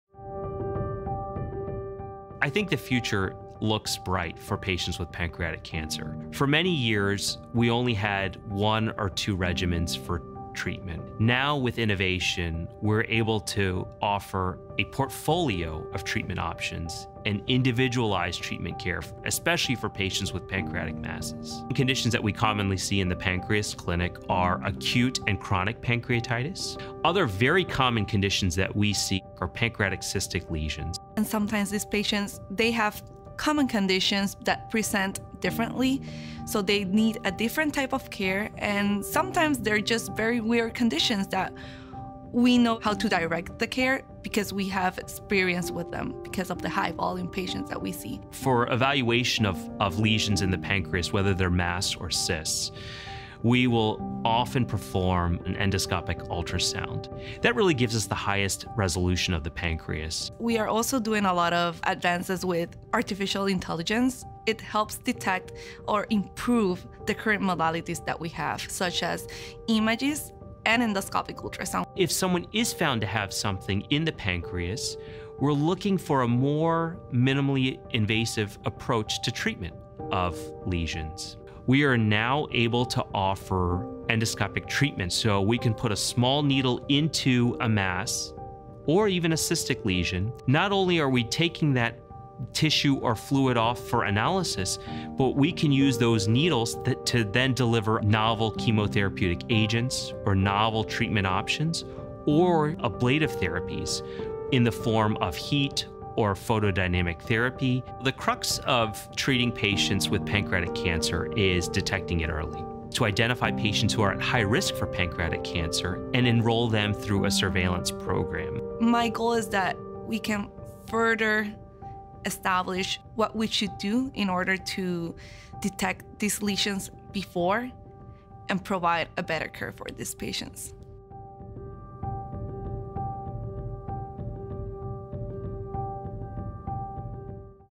Pancreas Clinic
Innovations in treatment for pancreatic cancer and chronic conditions within the pancreas give physicians a positive outlook for the future. By identifying patients at higher risk of issues, early treatment is possible and can lead to better outcomes.